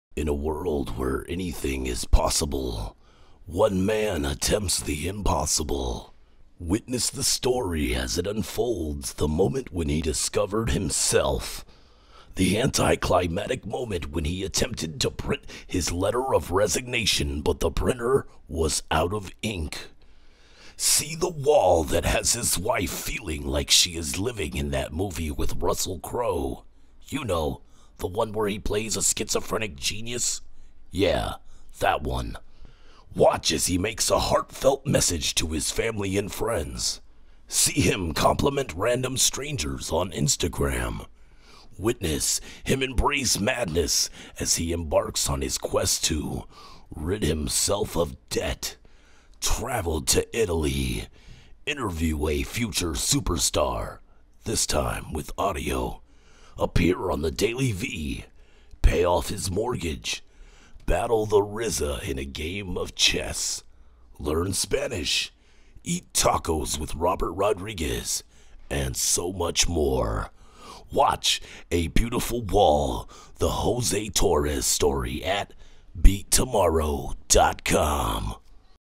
A Beautiful Wall - The Jose Torres Story - Trailer - BEAT tomorrow
People often say that what they're doing is going to be epic, legendary, or "a movie." If you're really doing something that's going to be a "movie," then make a trailer. That's what I did. This is my vision wall. I'm introducing my website and my goals for 2019. Enjoy. I was just having fun. Please don't sue me.